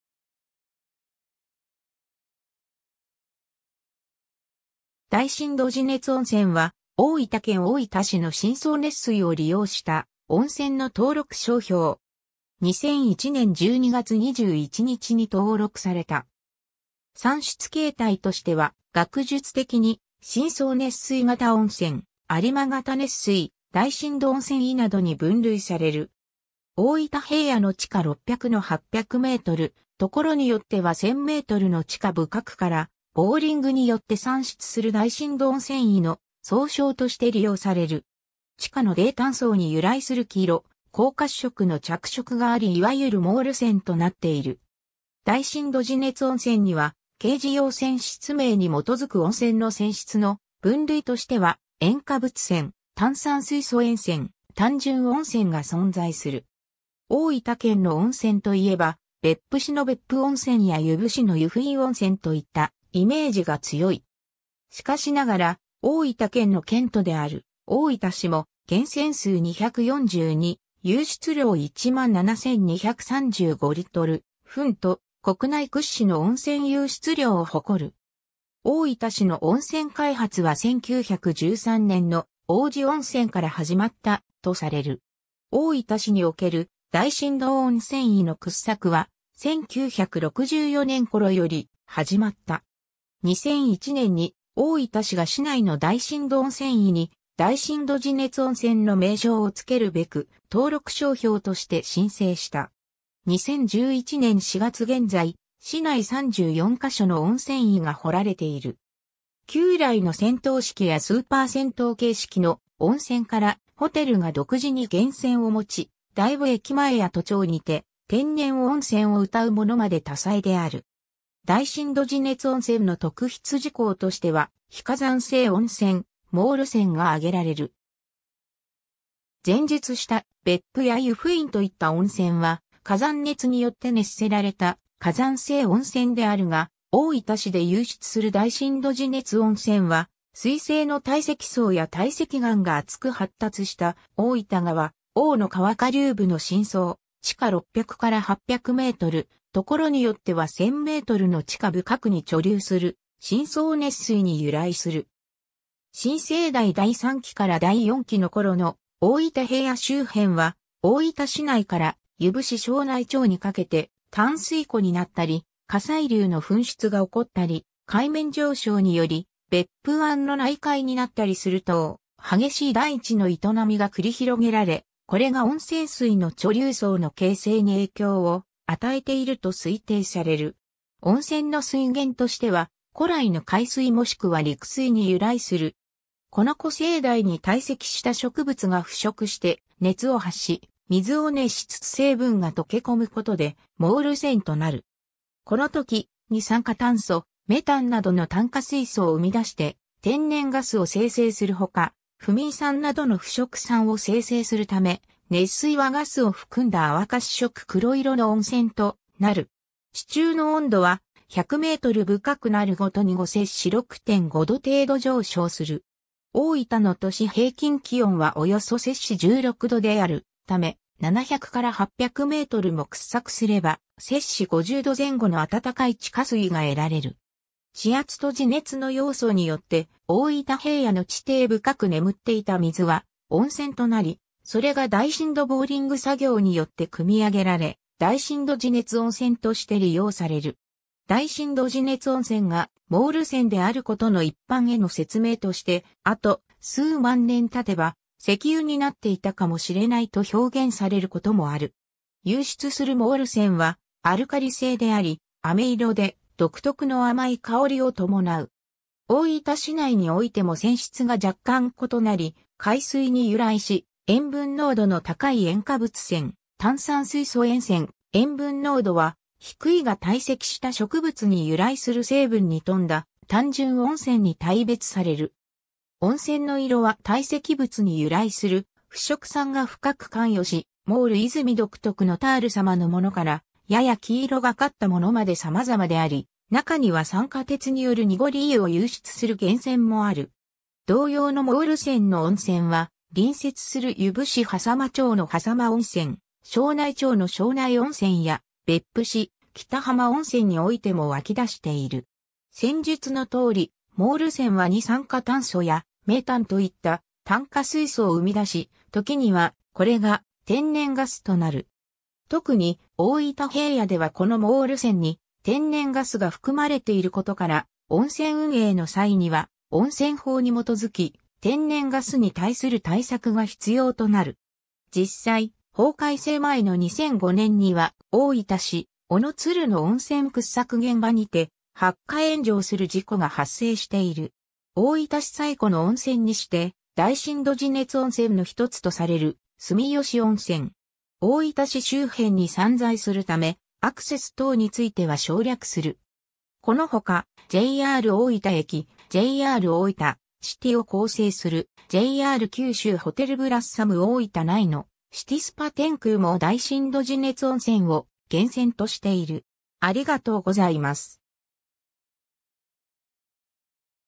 大深度地熱温泉
大深度地熱温泉（だいしんどちねつおんせん）は大分県大分市の深層熱水を利用した温泉の登録商標。
2001年12月21日に登録された。
産出形態としては学術的に深層熱水型温泉、有馬型熱水、大深度温泉井などに分類される。
大分平野の地下600-800m、所によっては1,000ｍの地下深くからボーリングによって産出する大深度温泉井の総称として利用される。
地下の泥炭層に由来する黄色-黄褐色の着色があり所謂モール泉となっている。
大深度地熱温泉には、掲示用泉質名に基づく温泉の泉質の分類としては、塩化物泉・炭酸水素塩泉・単純温泉が存在する。
 大分県の温泉といえば、別府市の別府温泉や由布市の由布院温泉といったイメージが強い。
しかしながら大分県の県都である大分市も源泉数242、湧出量17,235L/分と国内屈指の温泉湧出量を誇る。
大分市の温泉開発は1913年（大正13年）の王子温泉から始まった、とされる。
 大分市における大深度温泉井の掘削は、1964年（昭和39年）頃より始まった。
2001年に大分市が市内の大深度温泉井に「大深度地熱温泉」の名称を付けるべく登録商標として申請した。
2011年（平成23年）4月現在、市内34か所の温泉井が掘られている。
旧来の銭湯式やスーパー銭湯形式の温泉から、ホテルが独自に源泉を持ち、大分駅前や都町にて「天然温泉」を謳うものまで多彩である。
 大深度地熱温泉の特筆事項としては、「非火山性温泉」、「モール泉」が挙げられる。
前述した別府や湯布院といった温泉は火山熱によって熱せられた「火山性温泉」であるが、大分市で湧出する大深度地熱温泉は、水成の堆積層や堆積岩が厚く発達した大分川・大野川下流部の深層、地下600～800m、所によっては1,000ｍの地下深くに貯留する深層熱水に由来する。
新生代第三紀から第四紀の頃の大分平野周辺は、大分市内から由布市庄内町にかけて淡水湖になったり、火砕流の噴出が起こったり、海面上昇により別府湾の内海になったりする等、激しい大地の営みが繰り広げられ、これが温泉水の貯留層の形成に影響を与えていると推定される。
温泉の水源としては、古来の海水もしくは陸水（淡水）に由来する。
 この古生代に堆積した植物が腐食して熱を発し、水を熱しつつ成分が溶け込むことでモール泉となる。
この時、二酸化炭素、メタンなどの炭化水素を生み出して天然ガスを生成するほか、フミン酸などの腐食酸を生成するため、熱水はガスを含んだ淡褐色～黒色の温泉となる。
 地中の温度は、100ｍ深くなるごとに5-6.5℃程度上昇する。
大分の年平均気温はおよそ16℃であるため、700-800mも掘削すれば、50℃前後の温かい地下水が得られる。
地圧と地熱の要素によって、大分平野の地底深く眠っていた水は温泉となり、それが大深度ボーリング作業によって組み上げられ、大深度地熱温泉として利用される。
大深度地熱温泉がモール泉であることの一般への説明として、「あと数万年経てば（温泉水が）石油になっていたかもしれない」と表現されることもある。
 湧出するモール泉はアルカリ性であり、飴色で独特の甘い香りを伴う。
大分市内においても泉質が若干異なり、海水に由来し塩分濃度の高い塩化物泉・炭酸水素塩泉、塩分濃度は低いが堆積した植物に由来する成分に富んだ単純温泉に大別される。
温泉の色は堆積物に由来する腐食酸が深く関与し、モール泉独特のタール様のものから、やや黄色がかったものまでさまざまであり、中には酸化鉄によるにごり湯を湧出する源泉もある。
 同様のモール泉の温泉は、隣接する由布市挾間町の挾間温泉・庄内町の庄内温泉や、別府市北浜温泉においても湧出している。
 先述の通り、モール泉は二酸化炭素や、メタンといった炭化水素を生み出し、時にはこれが天然ガスとなる。
特に大分平野ではこのモール泉に天然ガスが含まれていることから、温泉運営の際には温泉法に基づき、天然ガスに対する対策が必要となる。
実際、法改正前の2005年には大分市小野鶴の温泉掘削現場にて発火炎上する事故が発生している。
 大分市最古の温泉にして、大深度地熱温泉の一つとされる住吉温泉。
 大分市周辺に散在するため、アクセス等については省略する。
 このほかJR大分駅「JRおおいたシティ」を構成する「JR九州ホテル ブラッサム大分」内の「シティスパてんくう」も大深度地熱温泉を源泉としている。